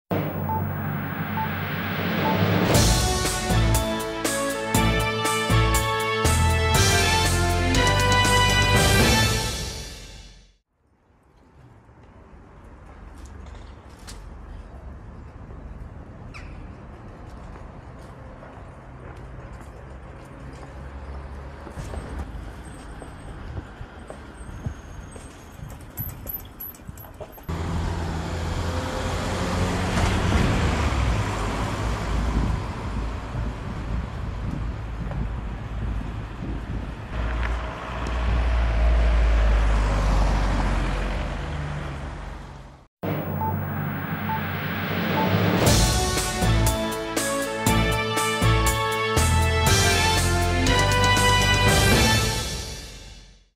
Nowe oznakowanie na ul. Wolności w Gołdapi
16 listopada 2023r. na ul. Wolności w Gołdapi pojawiło się nowe oznakowanie poziome.